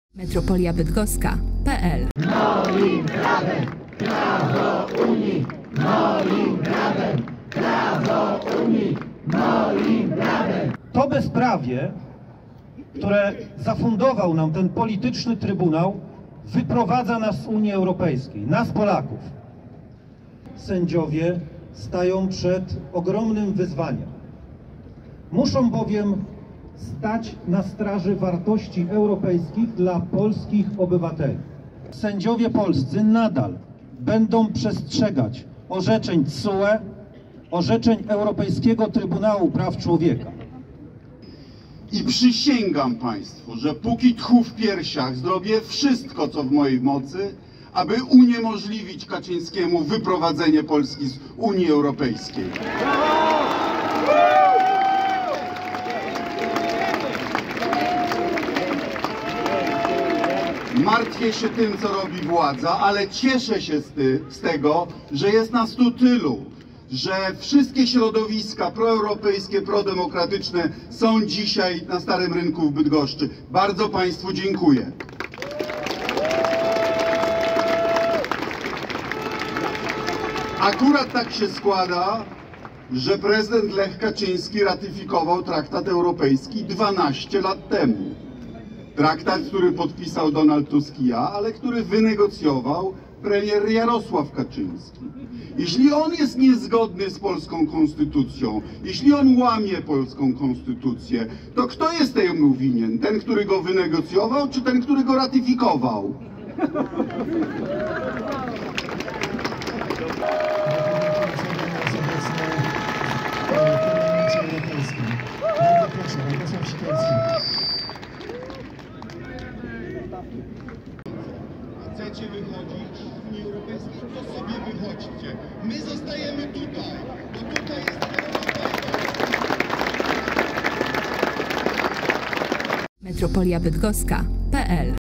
Manifestacja na Starym Rynku w Bydgoszcz [10.10.2021]
Nagranie z manifestacji przeciw wyjściu Polski z Unii Europejskiej, która odbyła się 10 października na Starym Rynku w Bydgoszczy.